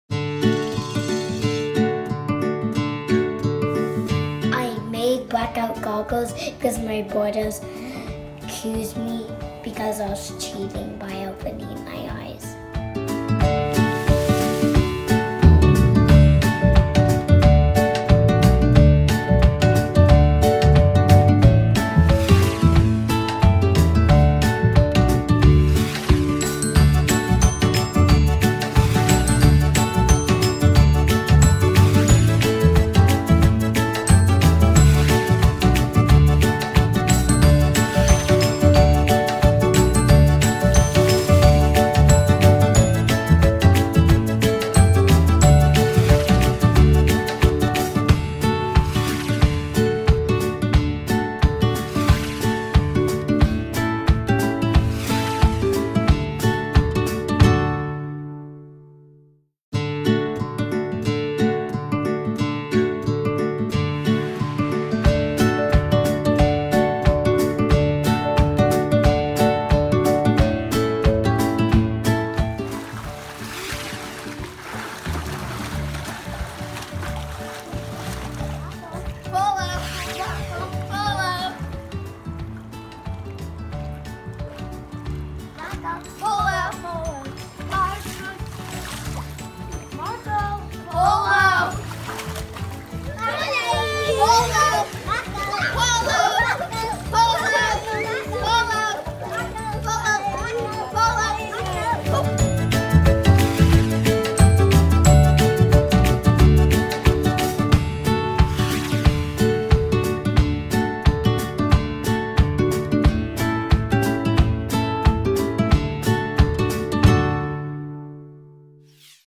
My Invention: Blackout Goggles
I was tired of my siblings accusing me of cheating at Marco Polo in the swimming pool. "He's opening his eyes" they would say. So I invented Blackout Goggles. Problem solved!